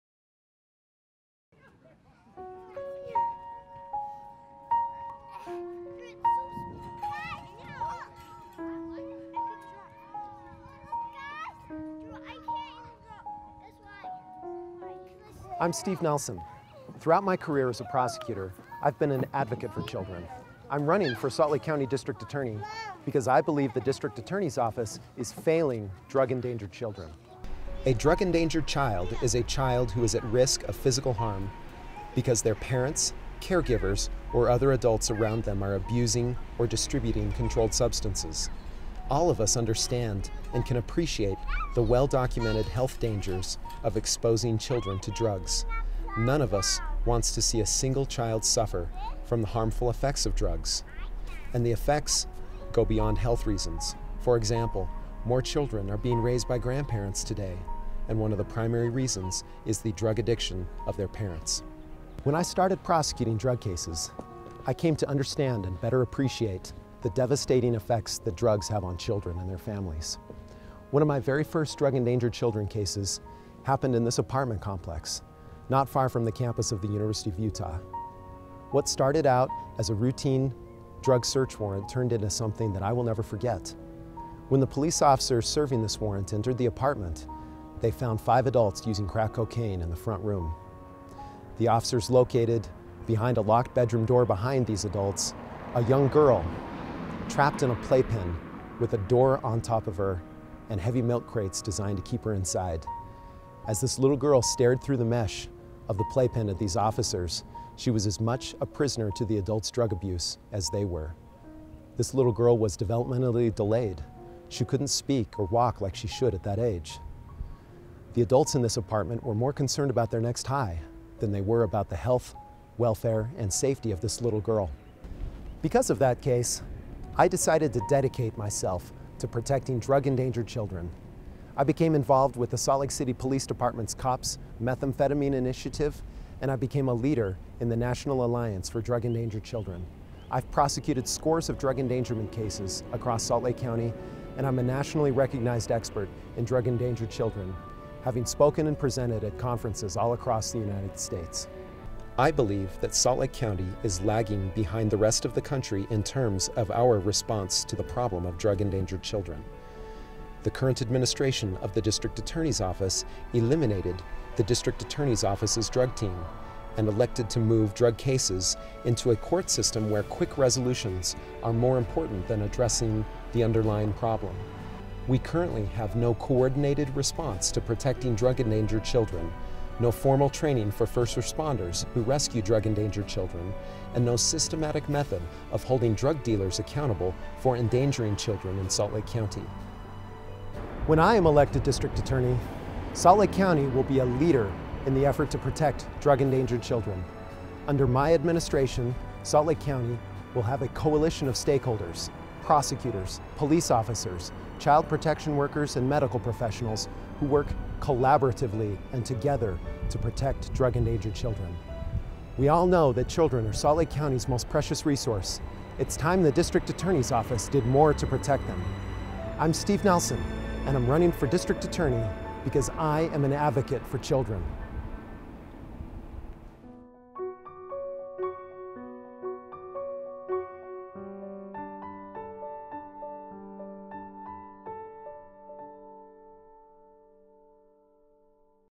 Steve Nelson for Salt Lake County District Attorney
Check out this short video on an important issue to me and what I will do to protect Drug Endangered Children when I am elected Salt Lake County District Attorney.